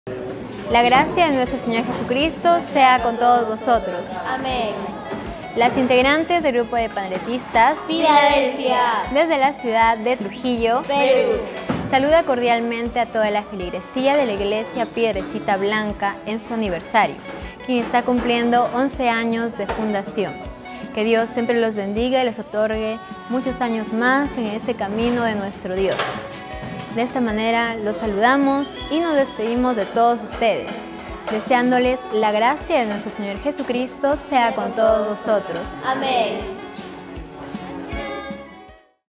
Saludo de las Panderetistas Philadelphia🌷 Aniversario de la ig. Piedrecita Blanca | AEMINPU TRUJILLO
🌷 Saludo del grupo de @PanderetistasPhiladelphia 💙 por el 11º Aniversario de la iglesia Piedrecita Blanca | Muy pronto celebrará su 12º aniversario 👏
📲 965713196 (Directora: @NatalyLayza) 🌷
📅 06/11/2022
🕔 04:54 a.m.